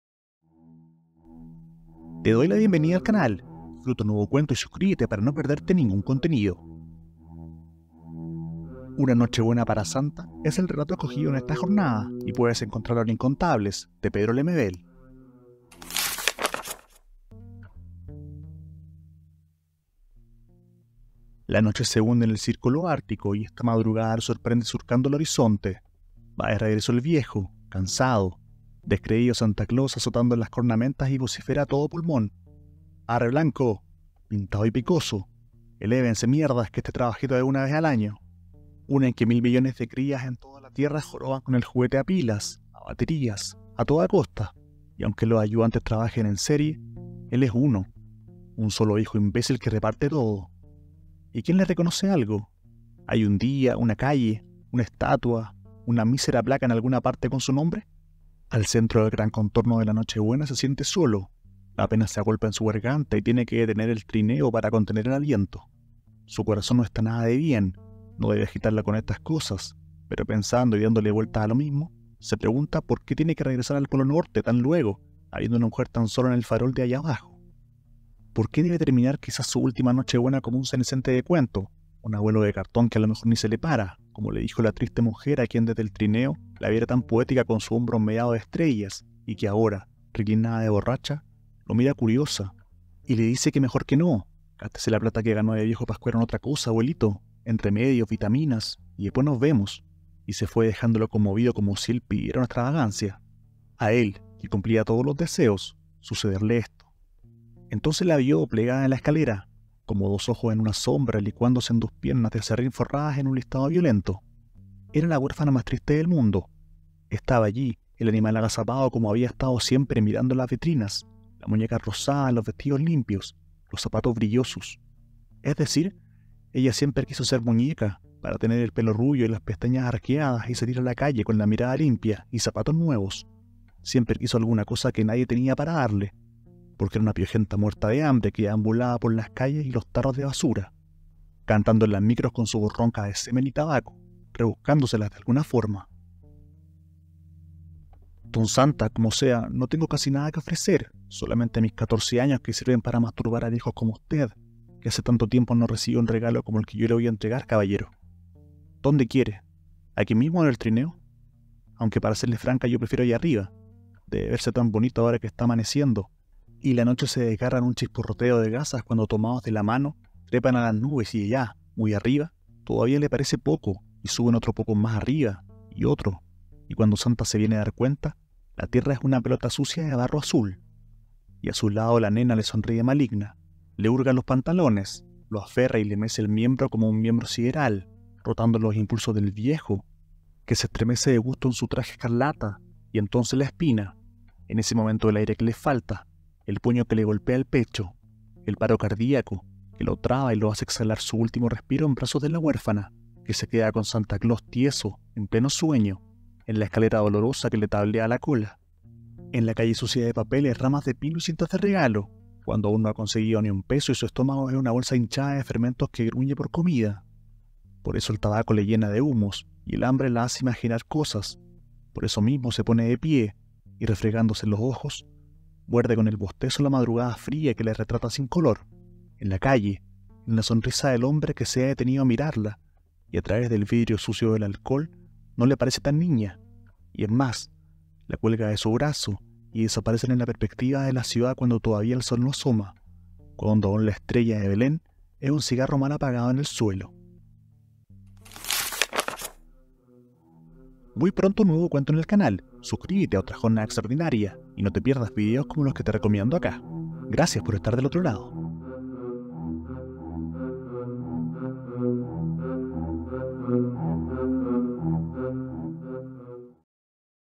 🎅 Una noche buena para Santa AUDIOLIBRO de Pedro Lemebel
🕣 Te doy la bienvenida a Otra Jornada Extraordinaria.

Regresan al canal los contenidos de Pedro Lemebel. "Una noche buena para Santa", relato que forma parte de Incontables (Seix Barral, 2018), nos trae al escritor en ciernes, a Mardones. Pero ya esa semilla tiene toda la ferocidad y desparpajo que le conoceríamos en los años siguientes.
Espero te guste el video, comenta qué autor te gustaría que apareciera en el canal.

 🟨 ¿COMENTARIOS O DUDAS? Estaré feliz de responderte en los comentarios
🕹 Mi nombre es Esteban Poblete ¡Gracias por pasar por el canal!